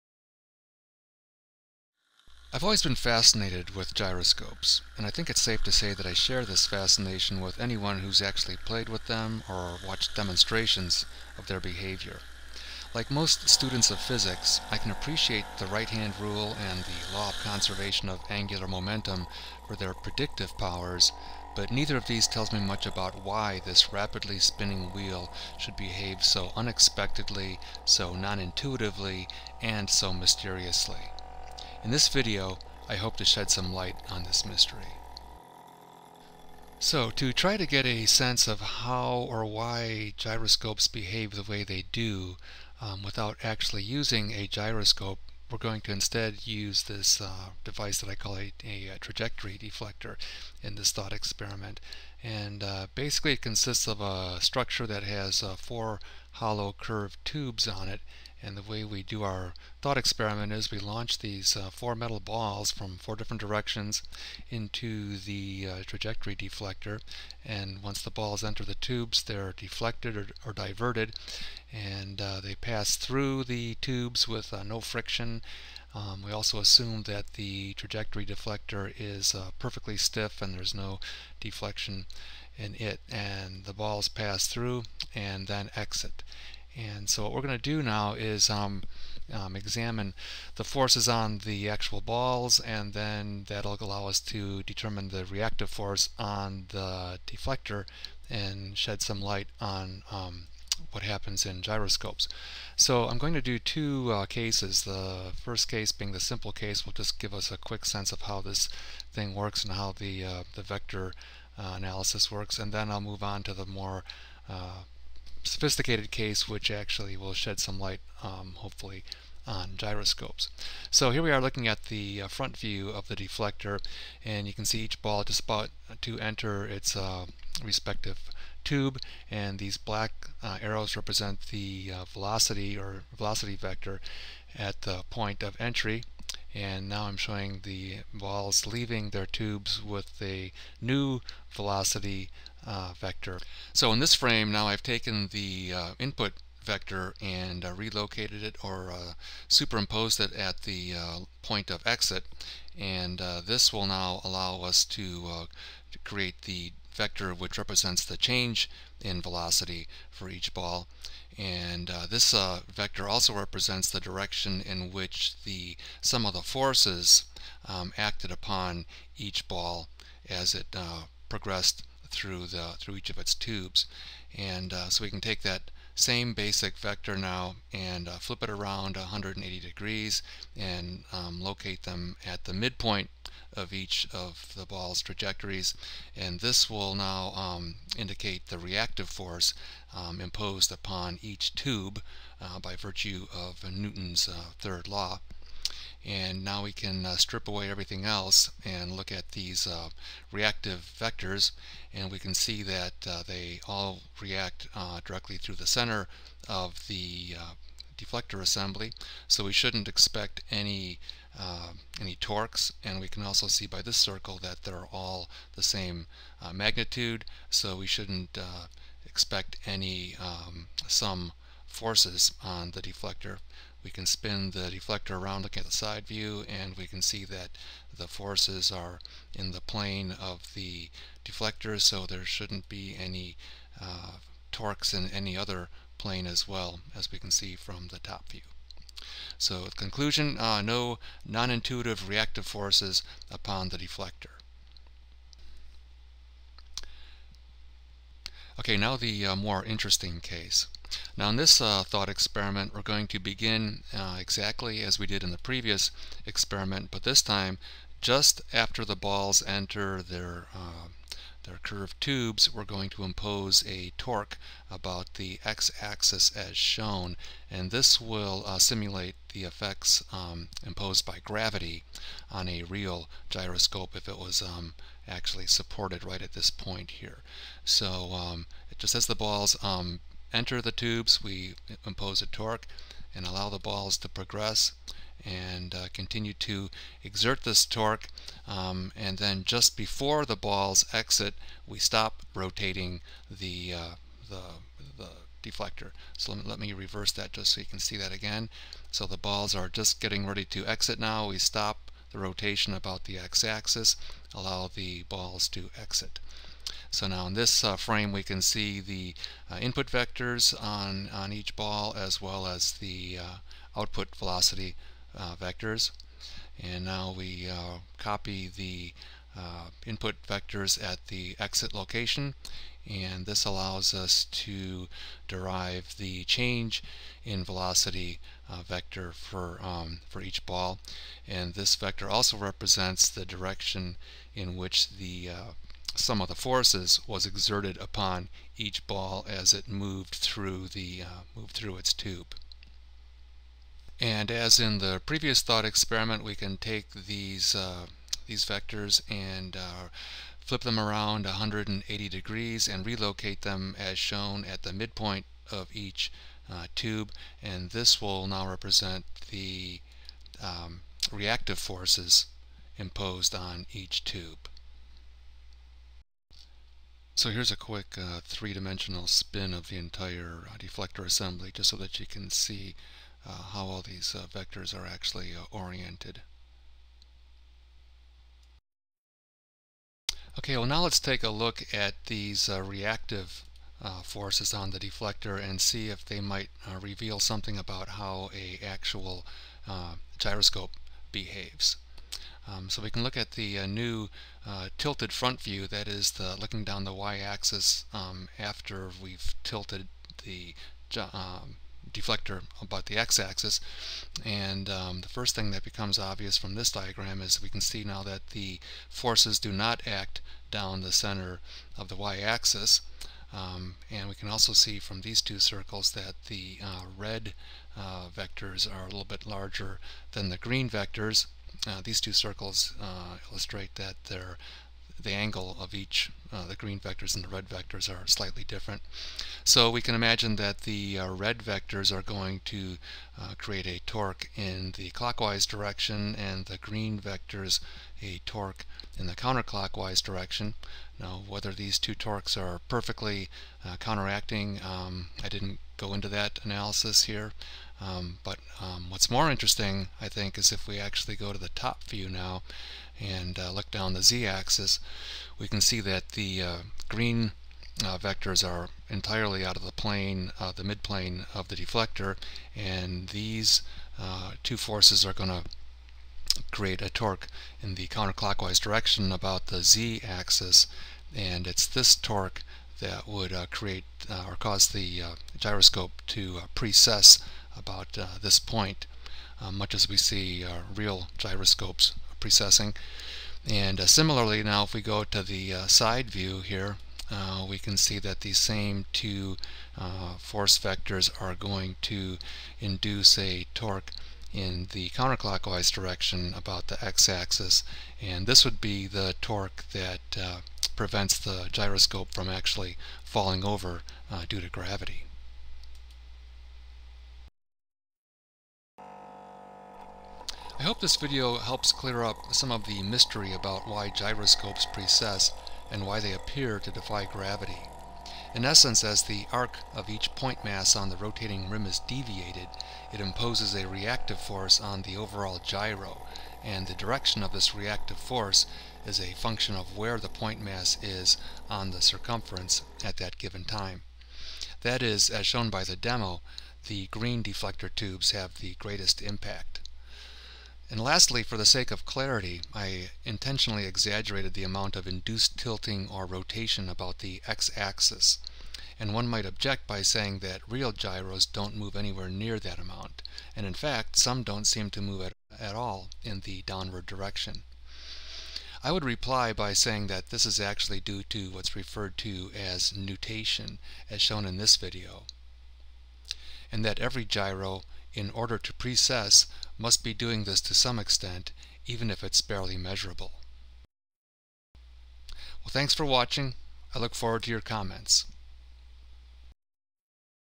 Solving the Mystery of Gyroscopes
This video illustrates why a gyro precesses  - and seems to defy gravity.